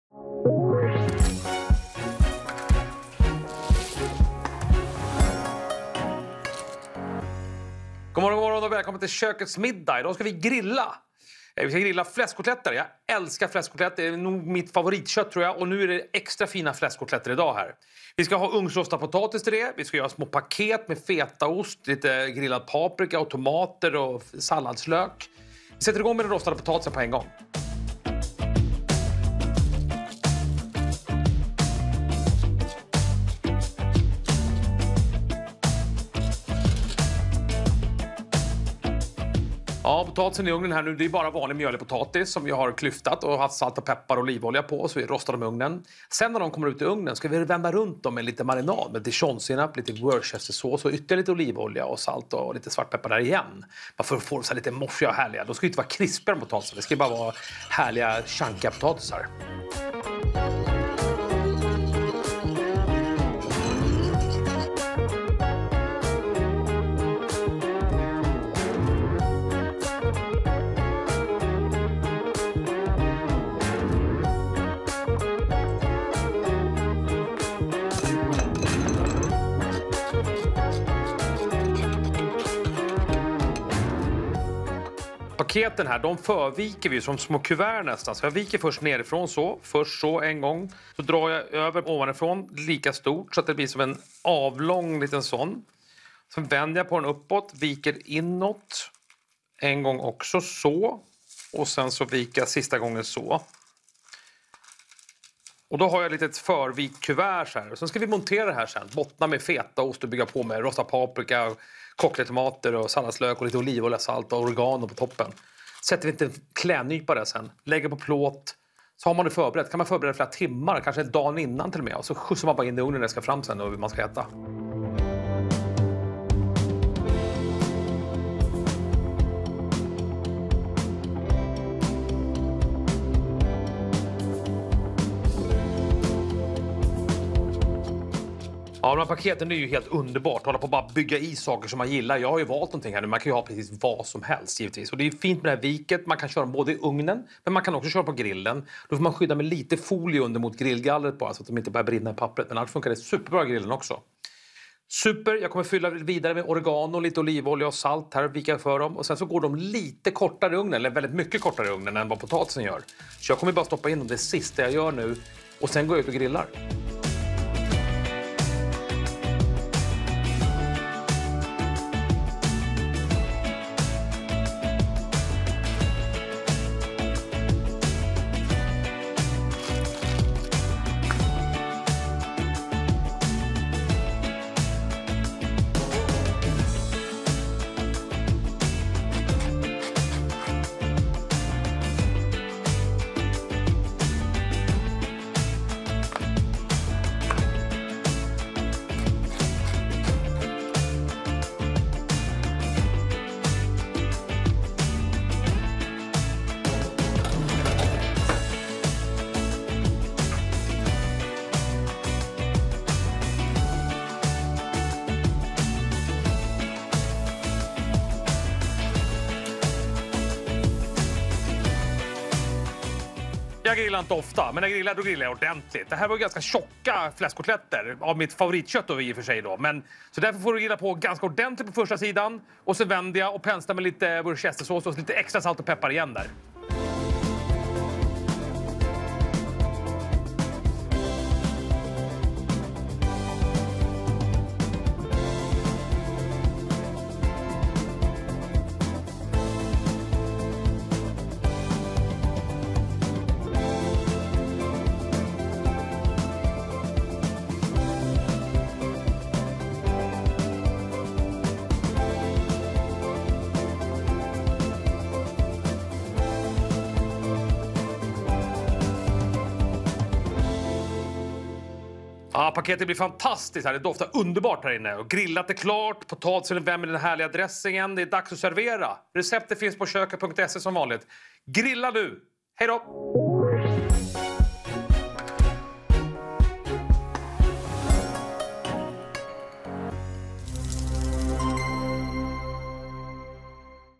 Kökets middag: Grillad fläskkotlett med fetaostpaket - Köket
Stefan Ekengren lägger kotletter på grillen och serverar med ugnsrostad potatis i god marinad och läckra paket fyllda med fetaost, tomat och paprika. Receptet hittar du hos köket.se